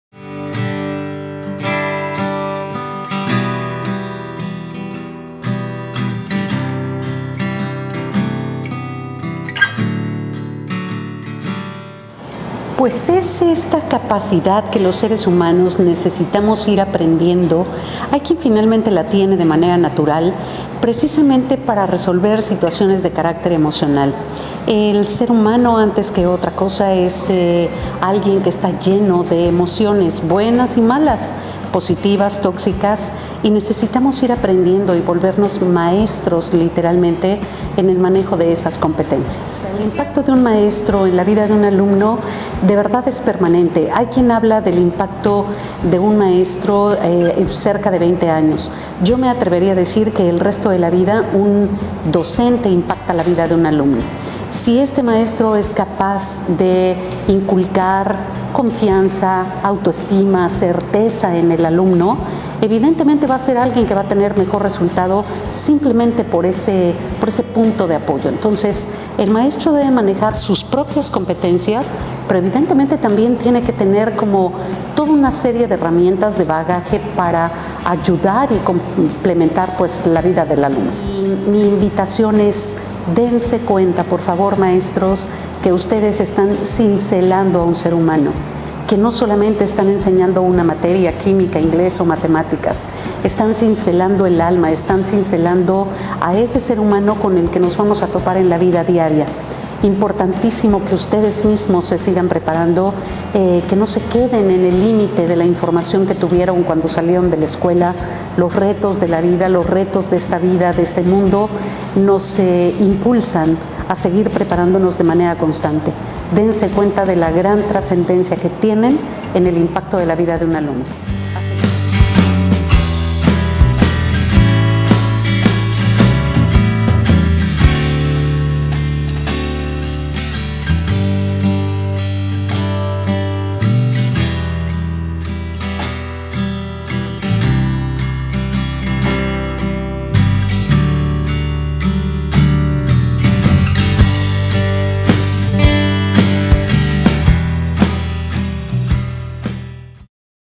Universidad Hispano Tuxtepec - Competencias Socioemocionales
La Universidad Hispano Campus Tuxtepec impartió el taller "Competencias Socioemocionales desde el docente hasta los estudiantes"